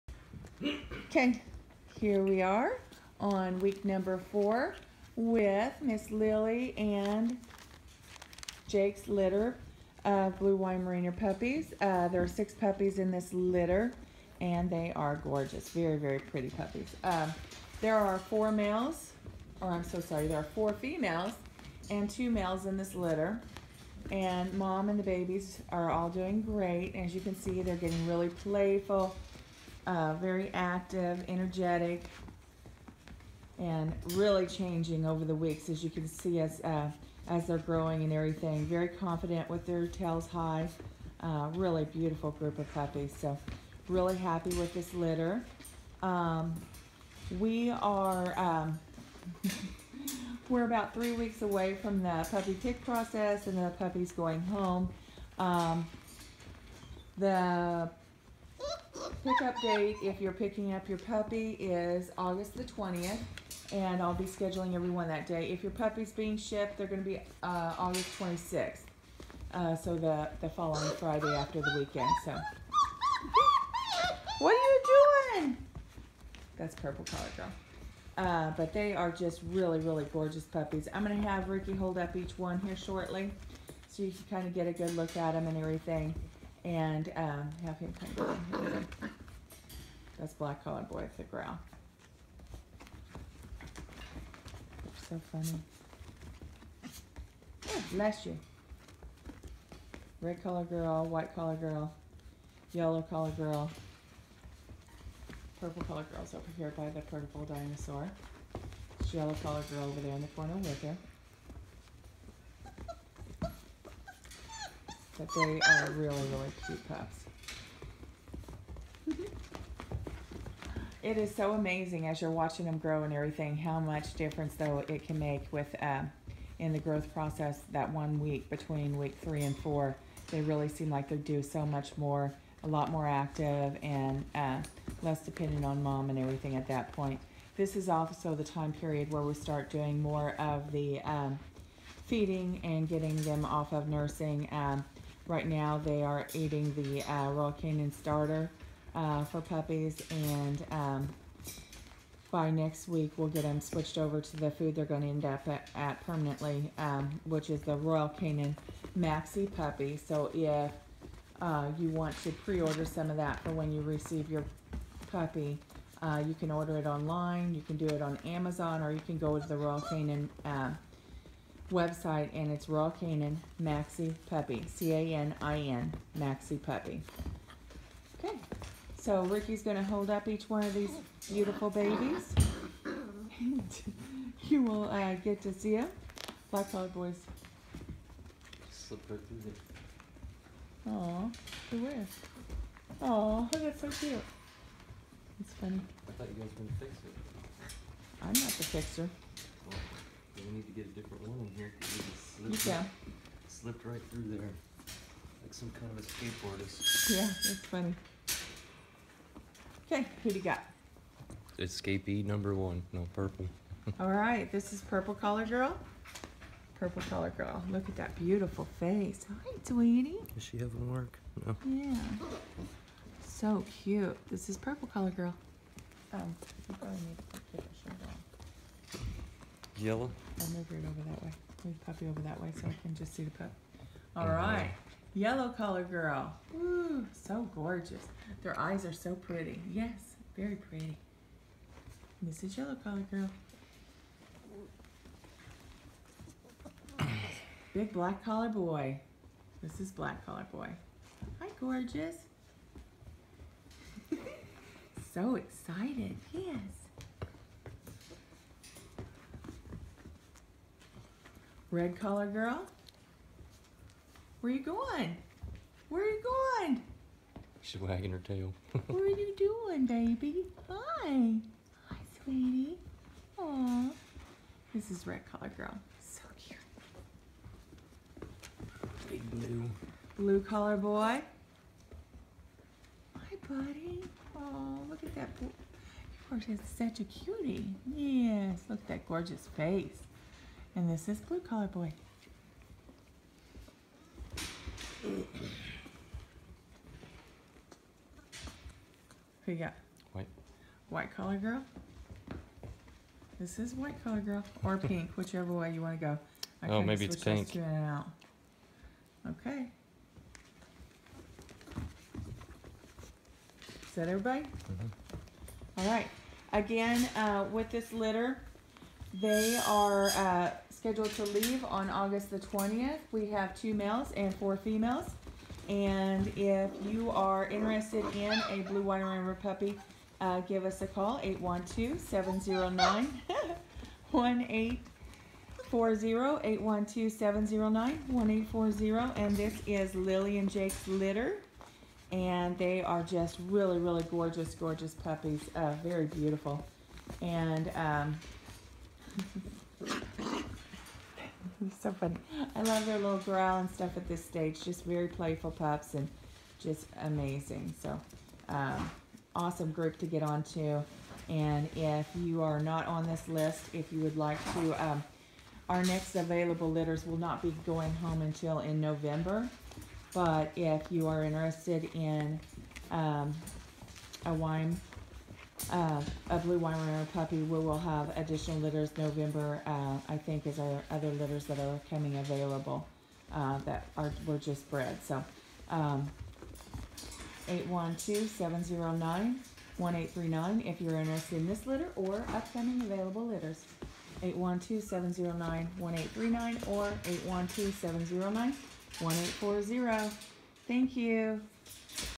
Lillys litter week 4
Lily and Jakes litter of blue Weimaraner puppy